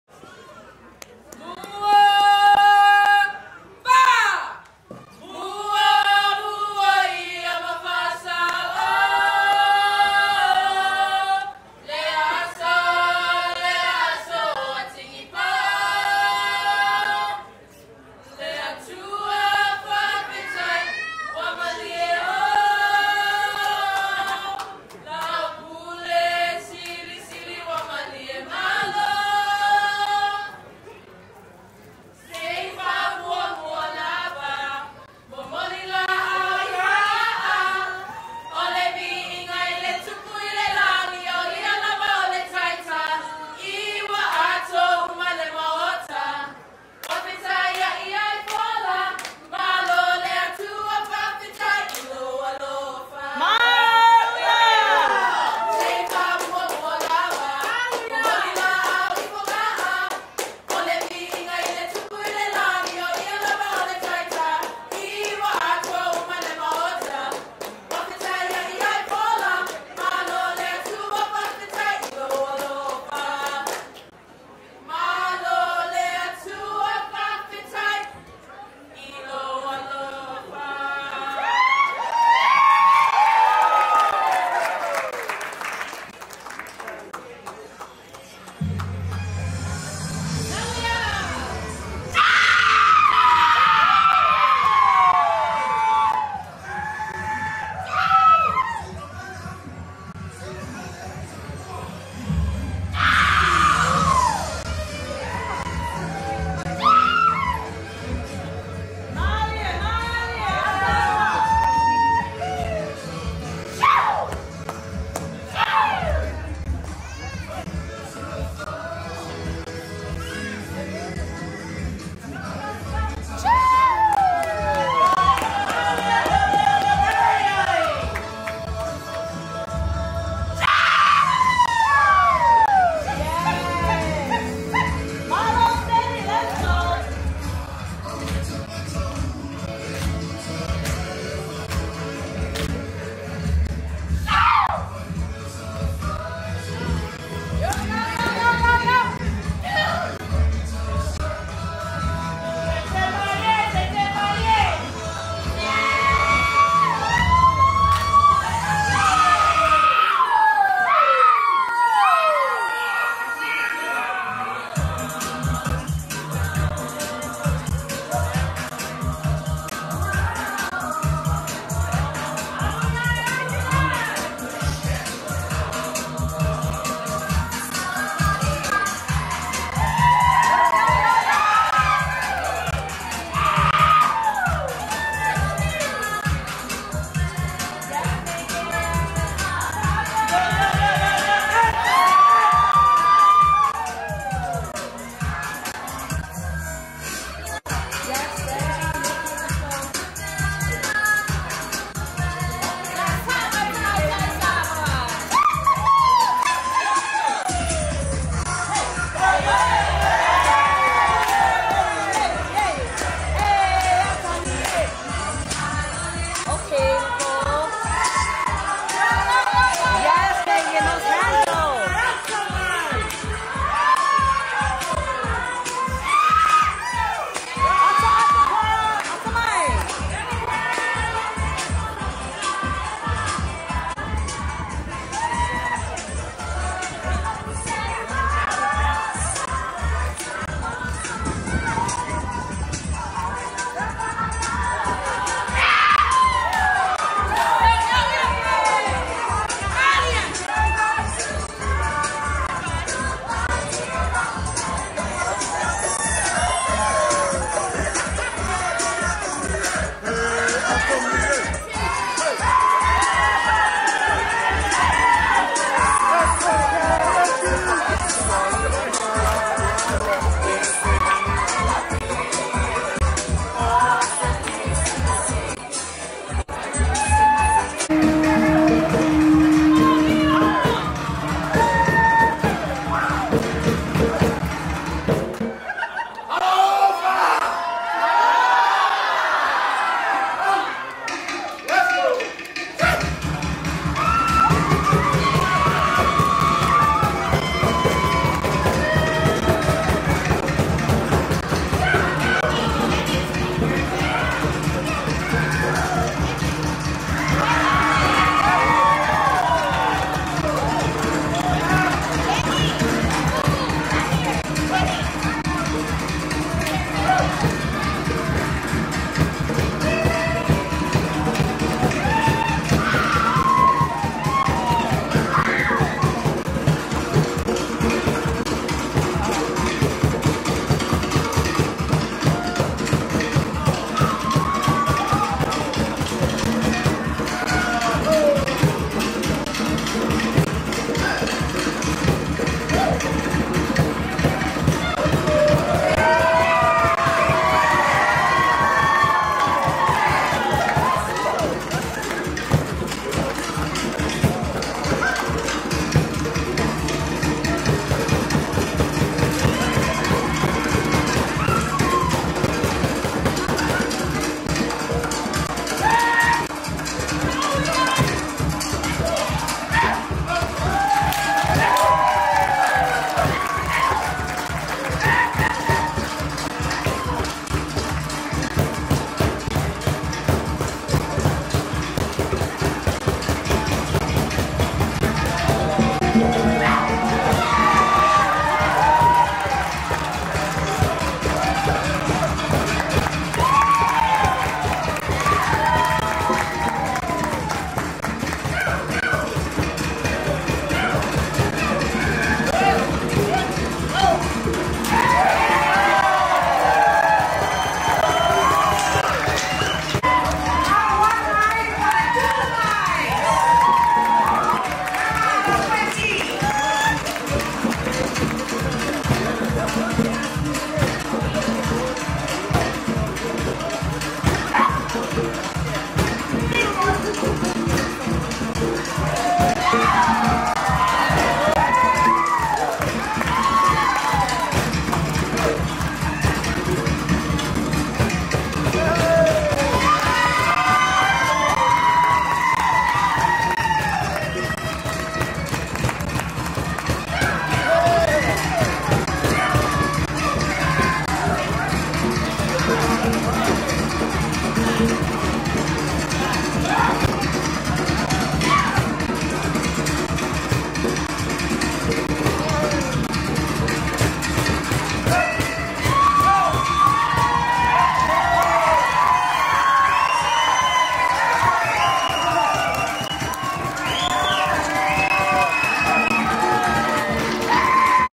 Siva Samoa and Siva Afi
A huge mahalo nui loa for Ashley fraser choreographing and teaching our family these dances to celebrate the Acero wedding. Fraser Cousin’s Siva for Rudy and Titilua’s wedding and Siva afi by Lopeti Tuua in Makawao, Maui 02/27/2021